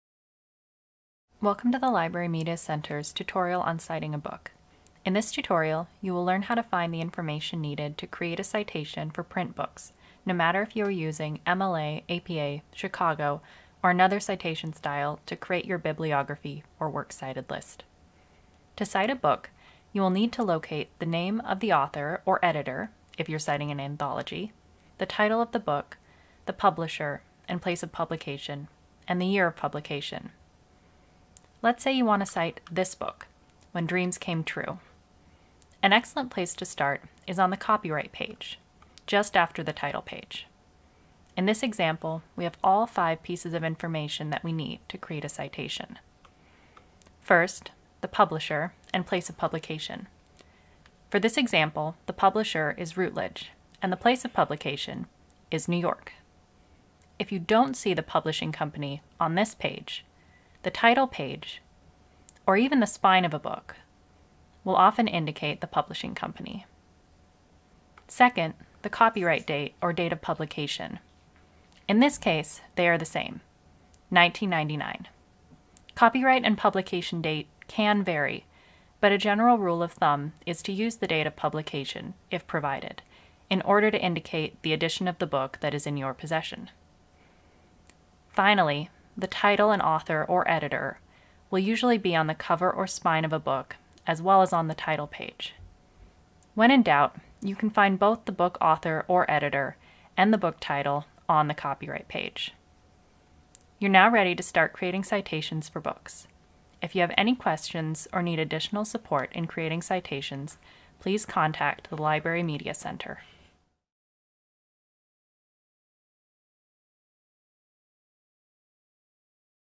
Citing a Book
A brief tutorial on finding the information needed to create a citation for a book.